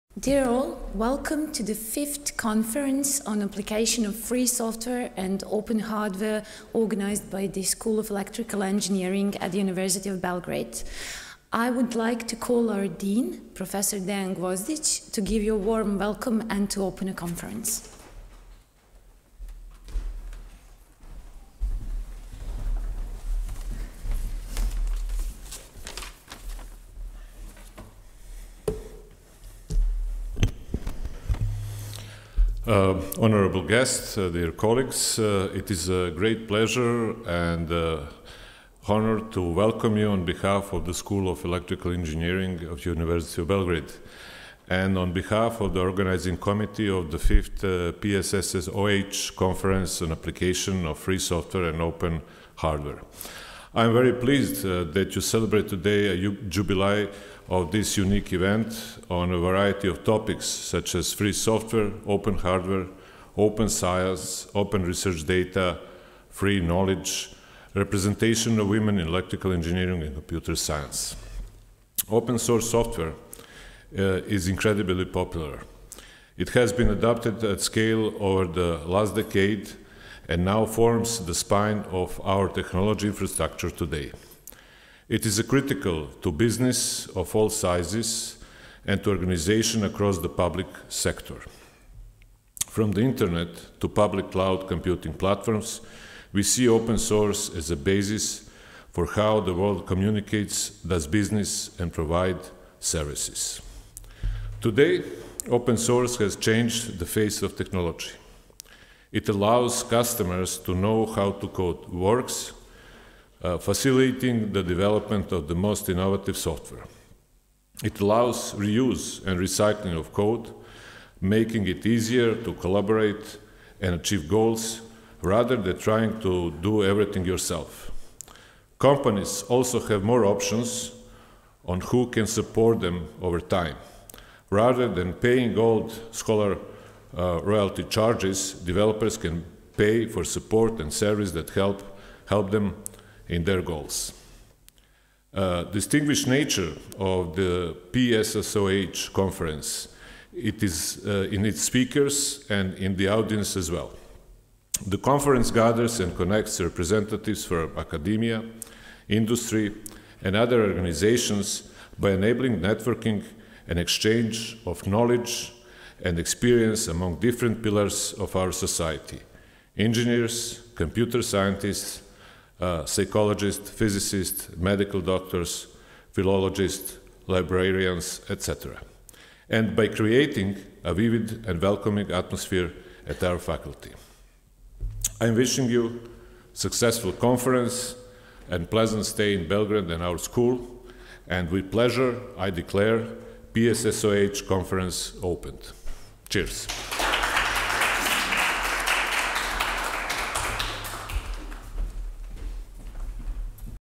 PSSOH 2022: Welcome note by ETF Dean
Official opening of the fifth PSSOH 2022 Conference by the Dean at the University of Belgrade - School of Electrical Engineering Professor Dejan Gvozdić on October 15, 2022.